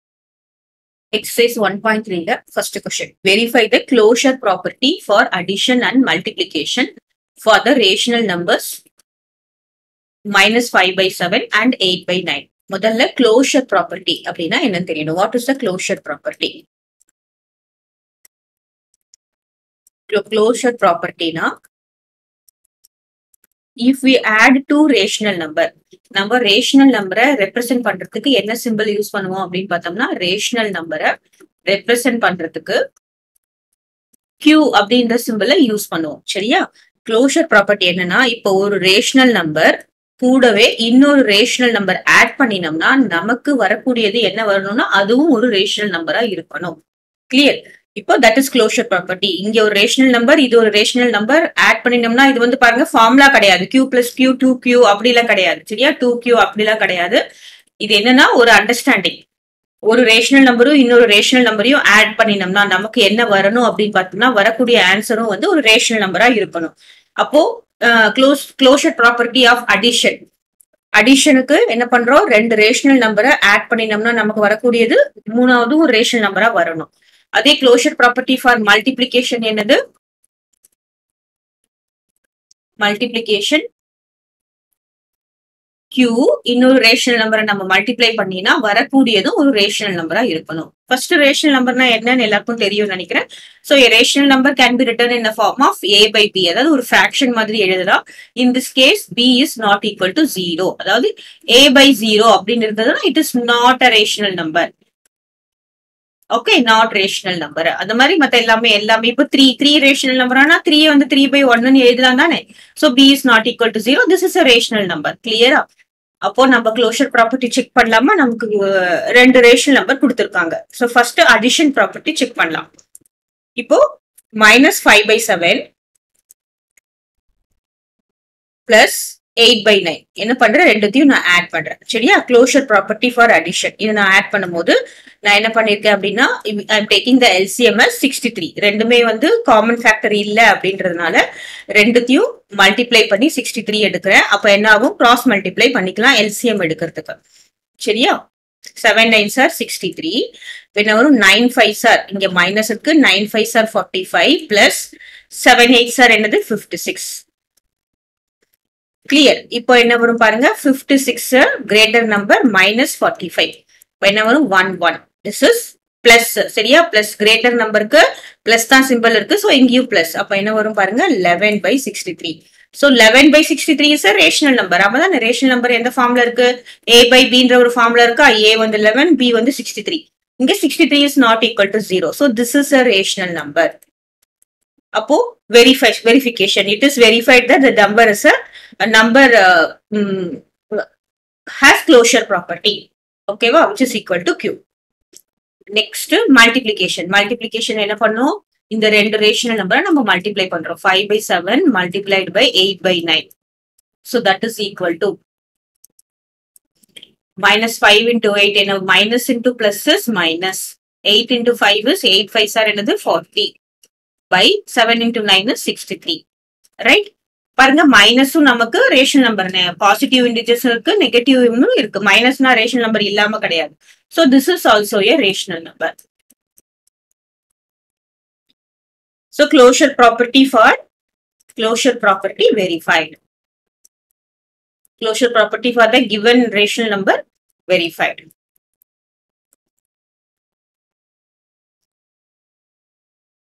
Class 8 TN Maths 1. RATIONAL NUMBERS Exercise 1.3 1. Verify the closure property for addition
TN 1. RATIONAL NUMBERS
 Exercise 1.3
1. Verify the closure property for addition and multiplication for the
rational numbers −5/7 and 8 /9